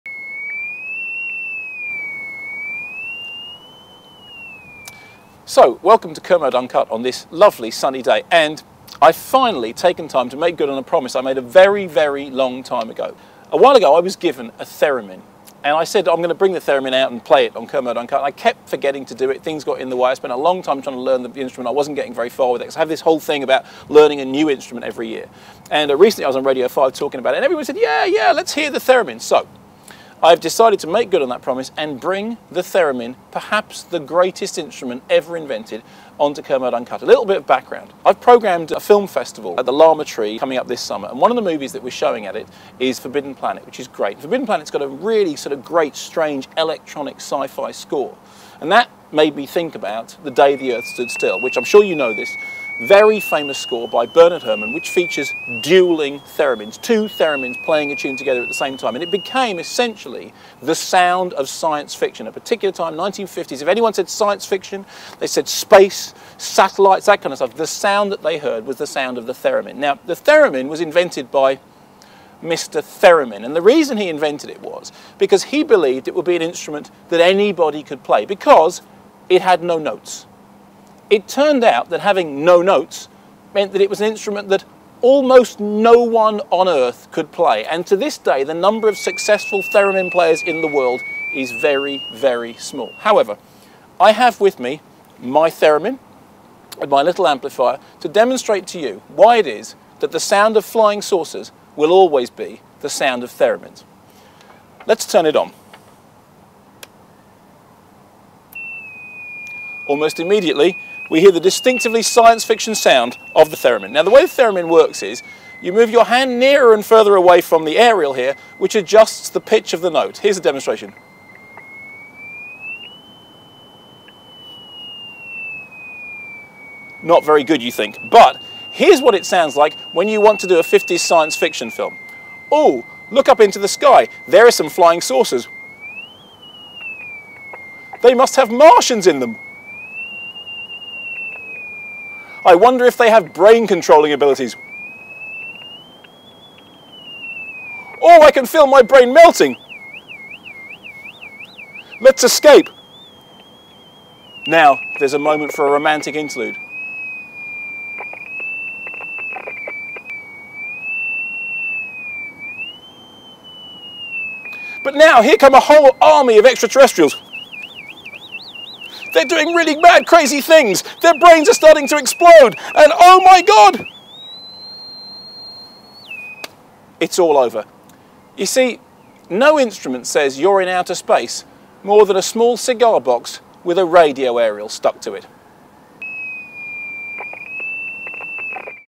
Kermode Uncut: Theremins
The Theremin is perhaps the greatest musical instrument ever invented and is the sound of 50s Sci Fi. Here I demonstrate its strange power...